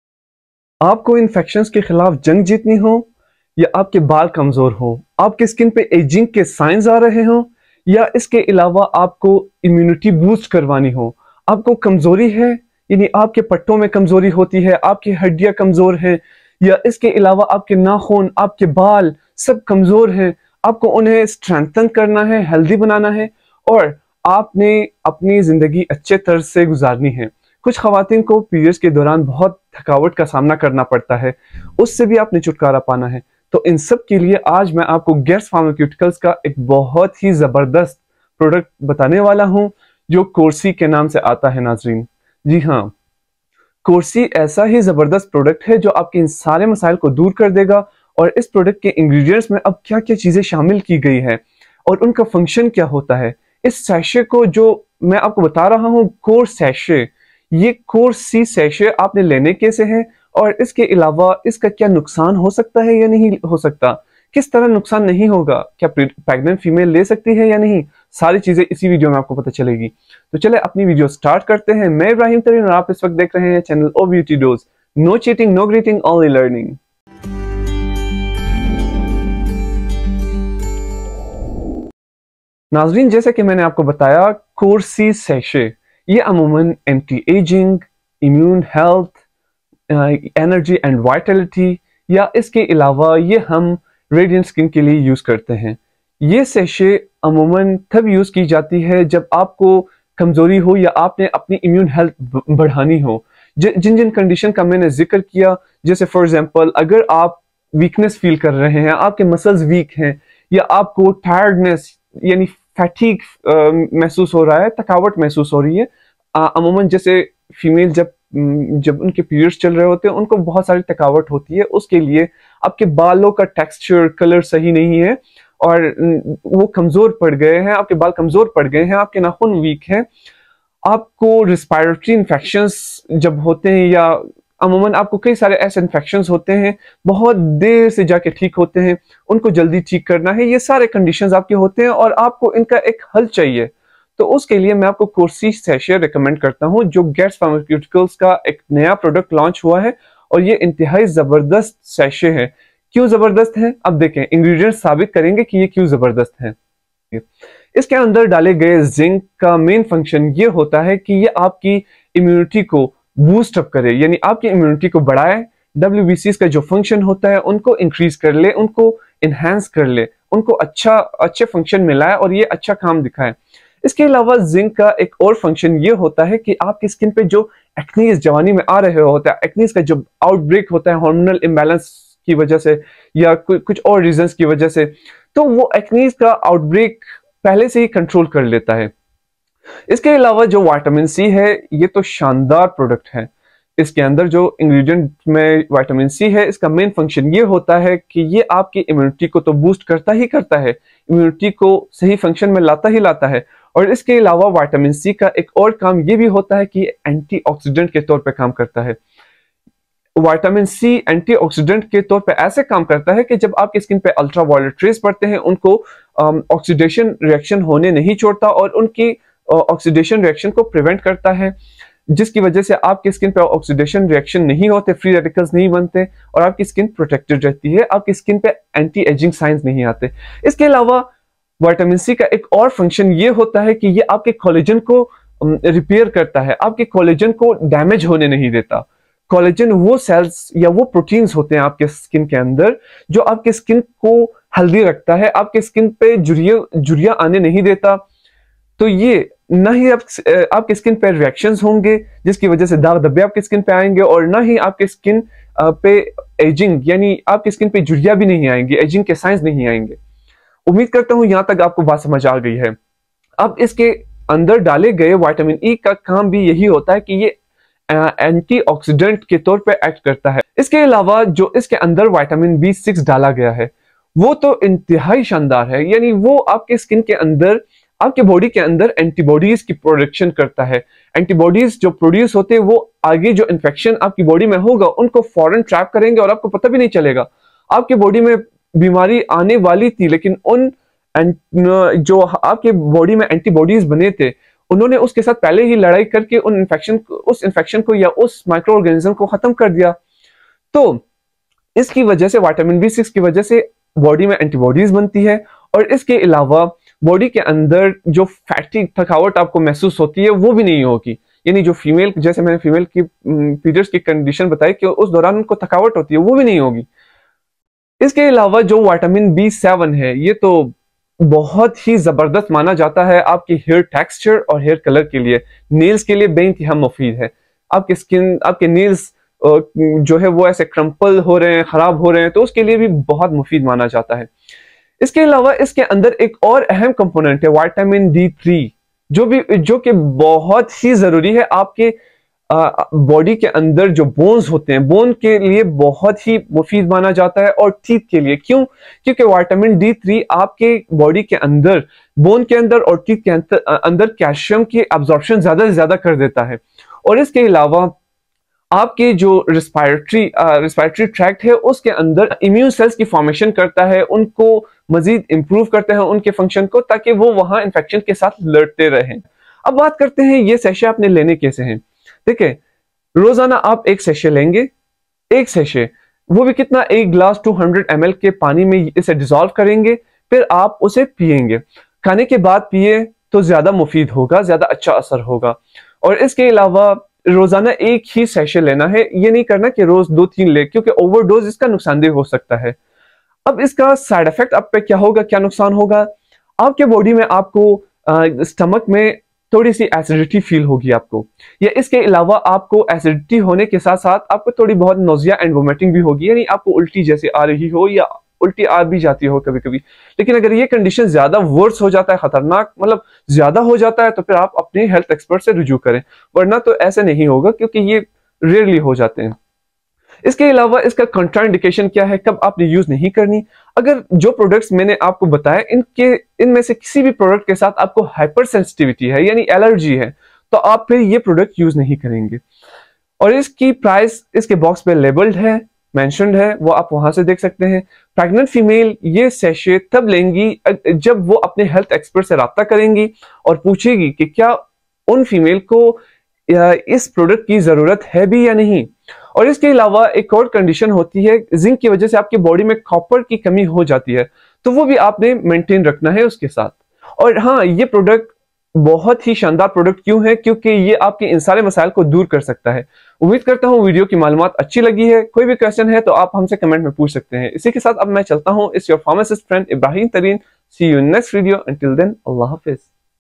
CORE C Sachet | Vitamin C 1000mg | Multivitamin | Getz Pharmaceuticals
Core C Sachet is an oral nutritional supplement with combination of 5 key Vitamins C, D, E, B6 & B7 (Biotin) and Elemental Zinc that play a central role in immunity and have potent antioxidant activity. Daily supplementation with Core C Sachet supports the function of the immune system. A strong immune system is needed to defend the body against a variety of infections. Moreover, the antioxidant properties of the ingredients contained in Core C Sachet prevents daily fatigue and aging via free radical scavenging. Antioxidants are also known to help maintain healthy skin.
Core C Sachet is free from sugar and sodium and hence suitable for diabetic and hypertensive patients.